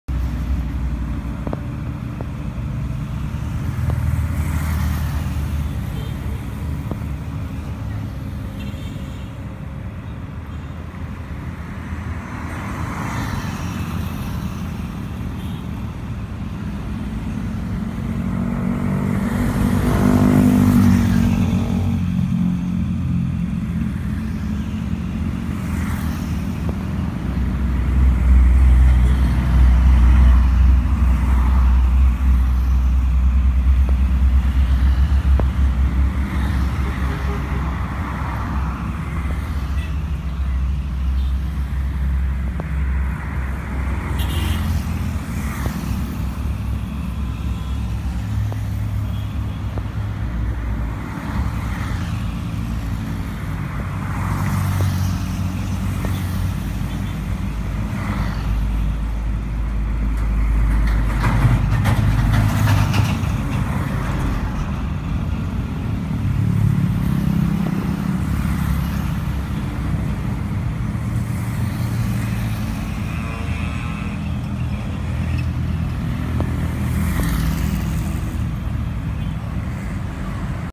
Tabrakan anak Pang dengan Pulisi
lakalantas ini diduga ketidaksengajaan anak pang yang banter dari arah gulon menabrak pengendara lain di dalanan